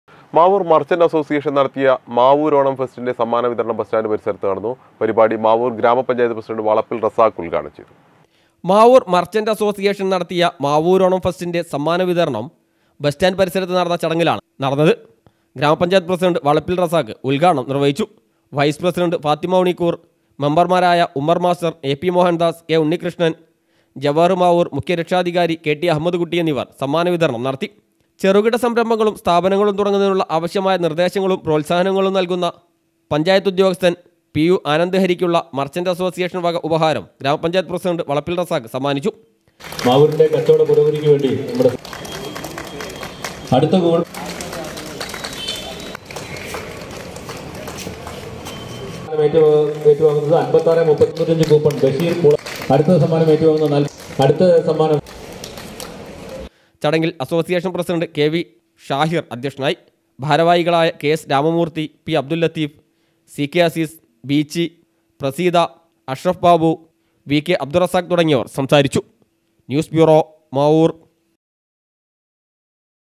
മാവൂരോണം ഫെസ്റ്റിന്റെ സമ്മാനവിതരണം ബസ് സ്റ്റാൻഡ് പരിസരത്ത് നടന്നു.
മാവൂർ മർച്ചന്റ് അസോസിയേഷൻ നടത്തിയ മാവൂരോണം ഫെസ്റ്റിന്റെ സമ്മാനവിതരണം ബസ് സ്റ്റാൻഡ് പരിസരത്ത് നടന്നു.